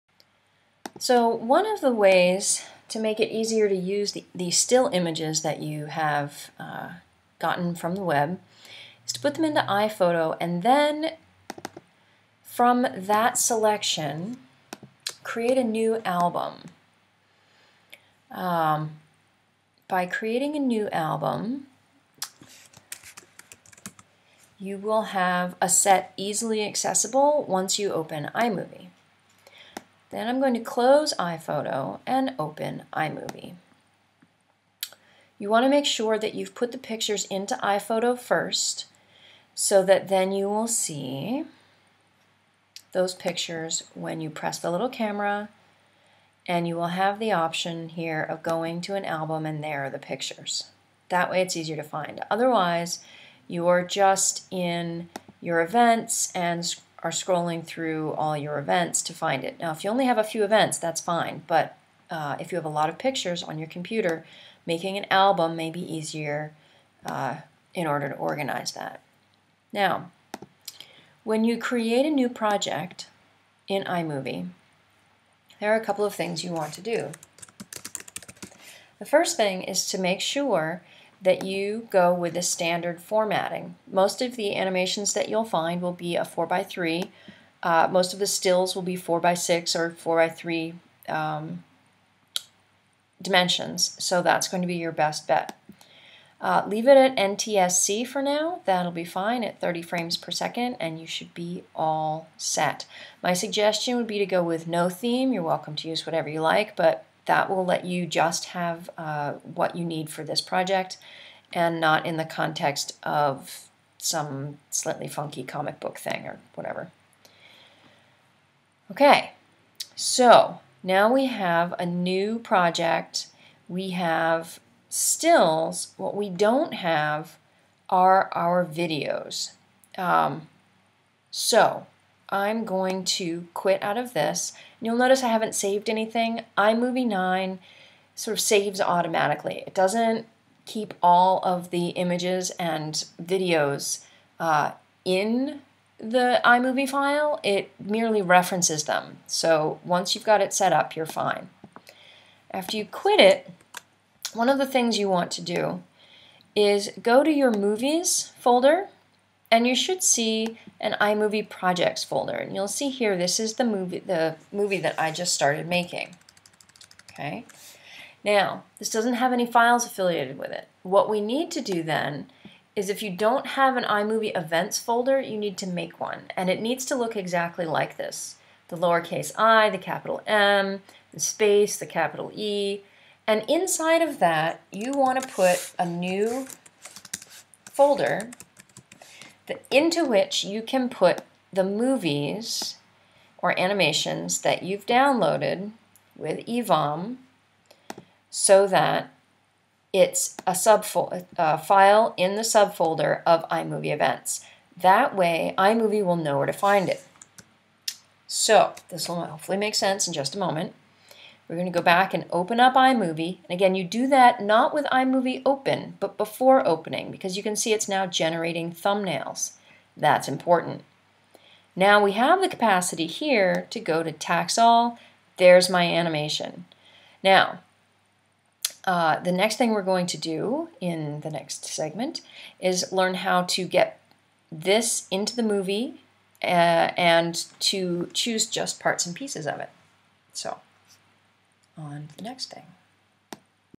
Movie 101c.m4v
Setting up iMovie events folder and organizing clips